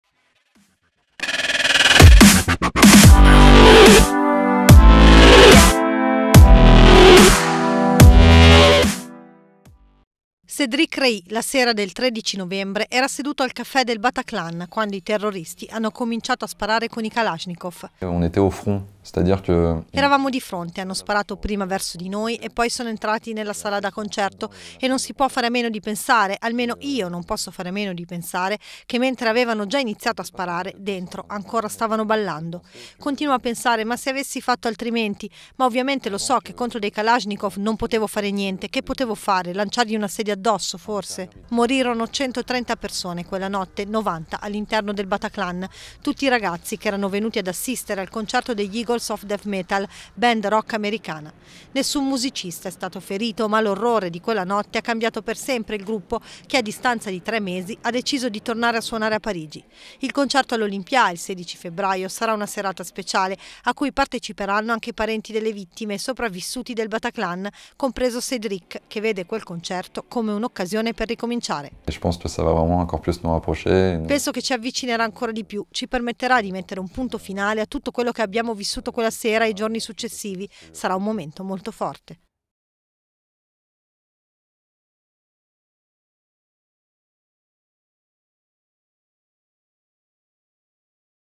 Parigi riparte, parla sopravvissuto del Bataclan (VIDEO)
In occasione di un nuovo concerto dei Eagles of Death Metal, la band che suonò al Bataclan di Parigi durante il tragico attentato, Cedric Rey, un sopravvissuto parla dell 'evento